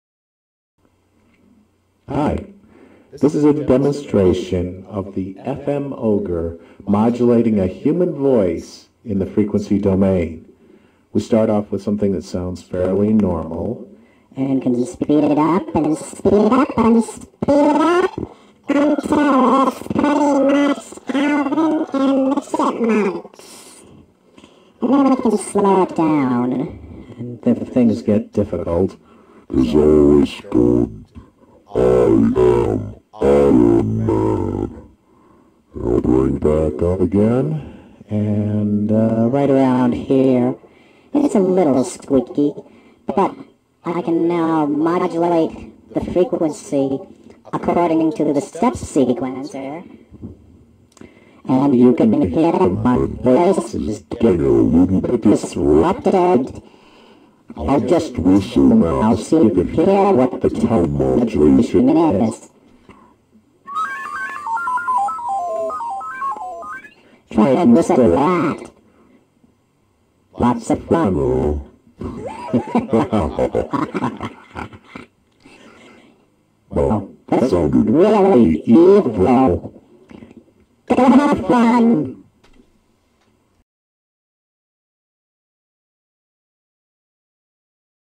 FMOgre Voice Video
Demo of an FM Ogre being used on human voice in realtime.   The FM Ogre is not just a high quality Thru-zero FM sinewave oscillator, it can do TZFM on _any_ incoming audio, including voice.

The FM Ogre does true pitch shifting - harmonics remain harmonically "in tune" with each other, unlike a Bode shifter that causes nonharmonic distortion.

The FM Ogre isn't perfect, but hey, it sounds pretty good to me.

Note- the audio here is live from a Crate keyboard amp, not wired in, so you will hear a tiny bit of feedback.  But at least this time, no My Little Pony in the background.

The audible soundpath here is JUST the FM Ogre. everything else in the chain was basically set to be a "copper wire with gain", no other distortion, filtering, etc. So what you hear is just me and the FM Ogre.